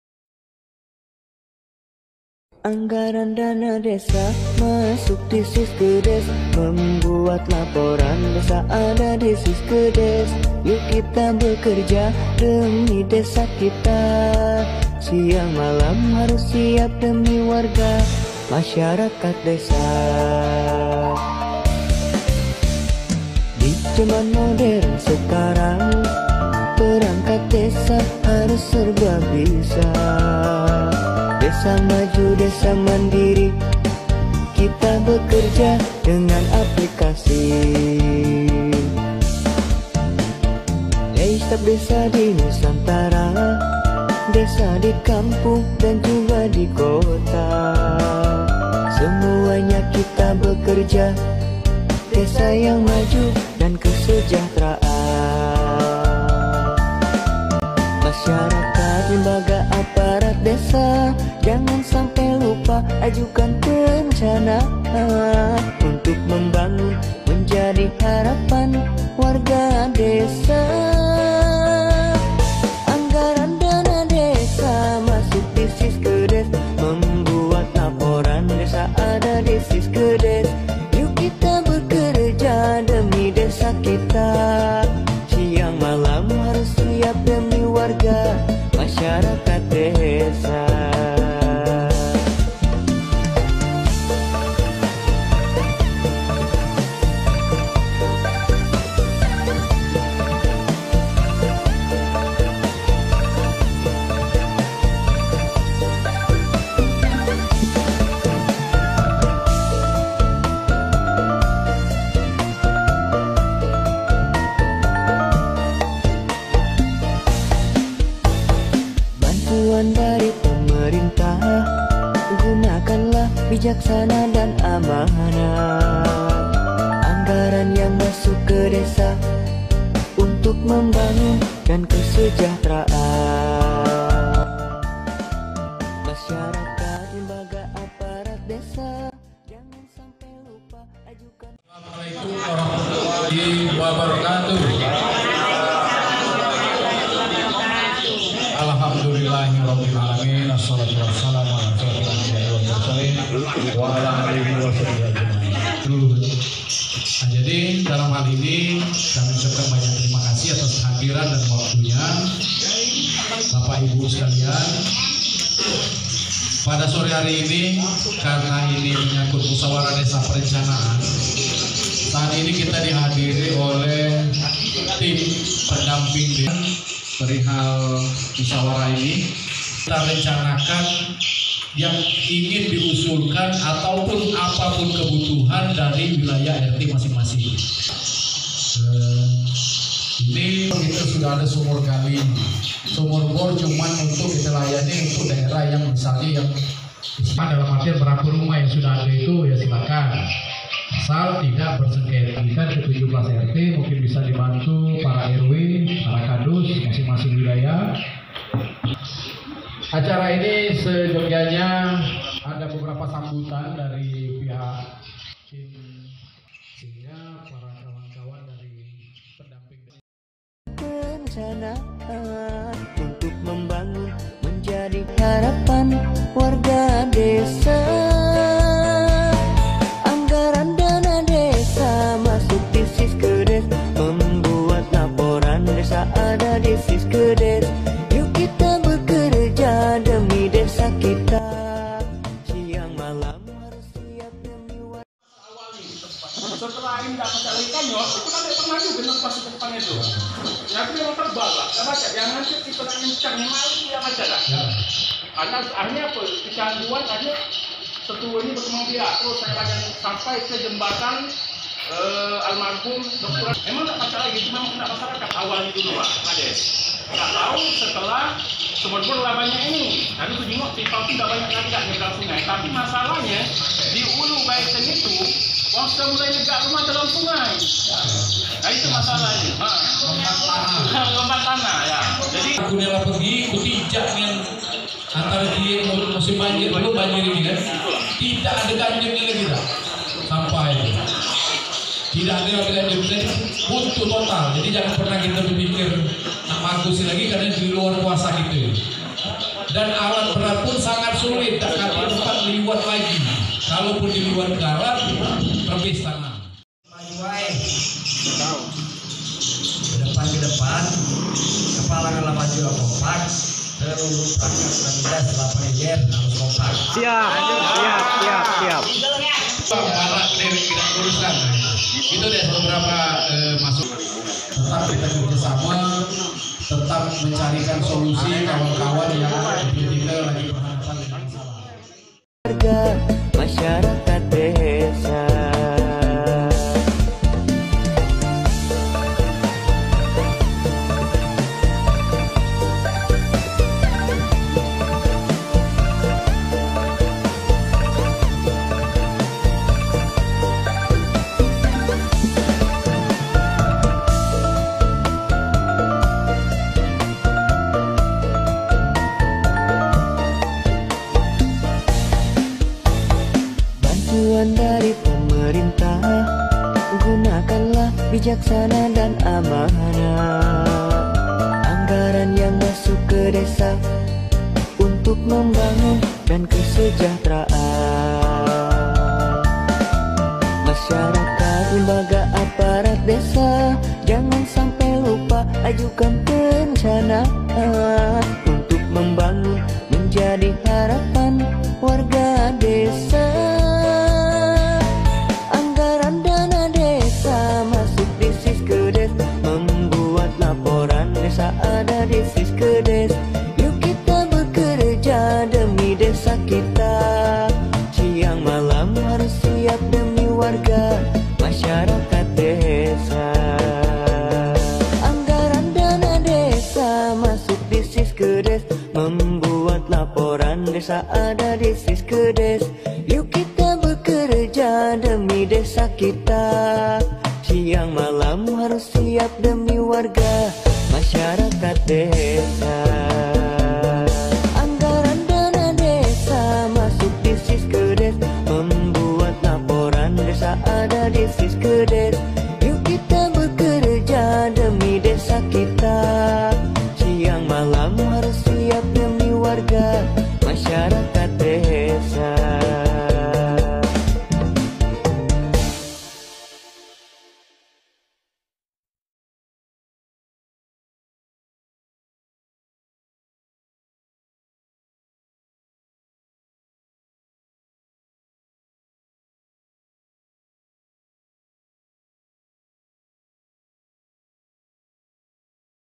RAPAT KOORDINASI ANTAR PEMERINTAH DESA Tulung selapan Ilir.
RAPAT KOORDINASI ANTAR PEMERINTAH DESA Tulung selapan Ilir,kecamatan Tulung selapan kabupate ogan Komering Ilir provinsi Sumatra Selatan.

Thanks udah nonton videoku, kalian boleh membagikan video ini ke temen-temen kalian, karena berbagi itu kebaikan, dan dapet pahala jugakan.."